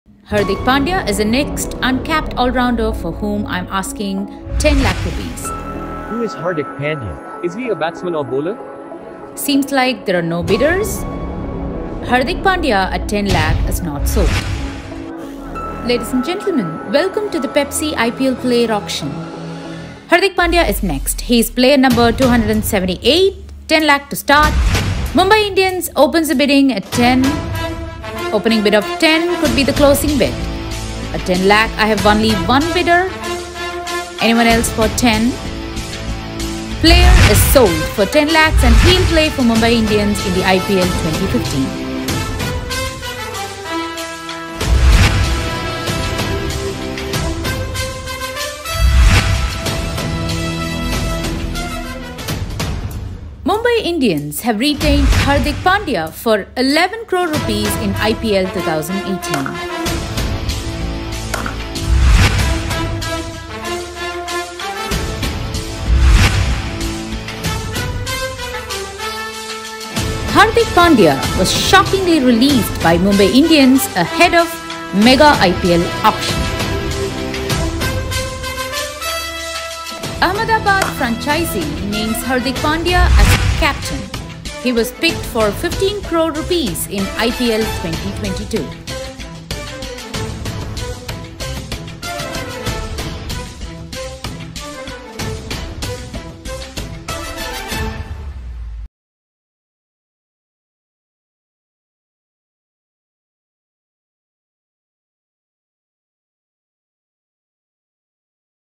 IPL 2024 Auction Live Ft. Hardik Pandya | IPL Mega Auction 2022 Updates
IPL 2022 MEGA Auction Live Updates : Hardik Pandya picked as captain by Ahmedabad Team in the IPL 2022. Few years back, Hardik Pandya was unsold in the IPL 2014, Later he was picked by Mumbai Indians for just 10 Lakhs in the IPL 2015 Auction. His team Mumbai Indians won IPL 2015, IPL 2017. In IPL 2018, Hardik Pandya was retained by Mumbai Indians for 11 Crores.

Photos / Video Courtesy : BCCI / IPL
Thumbnail Image : Sports Gully ( Instagram Page )